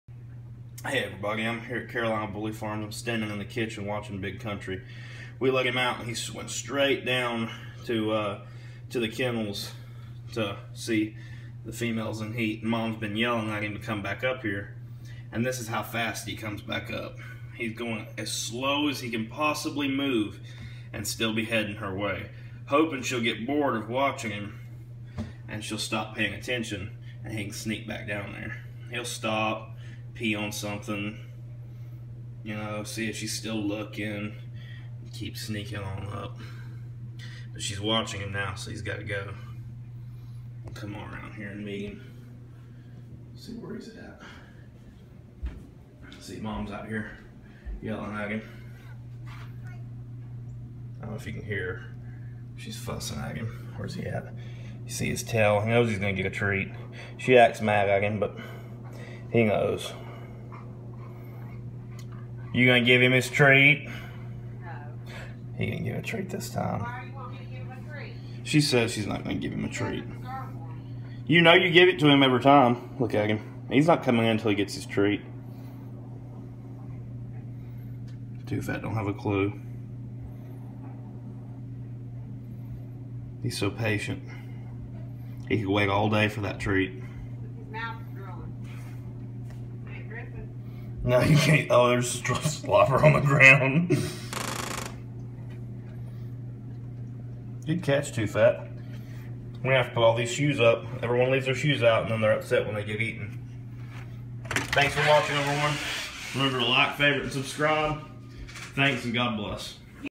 Big country trying to be sneaky
Big country thought he was being sneaky trying to see the females at Carolina bully farms Moneyline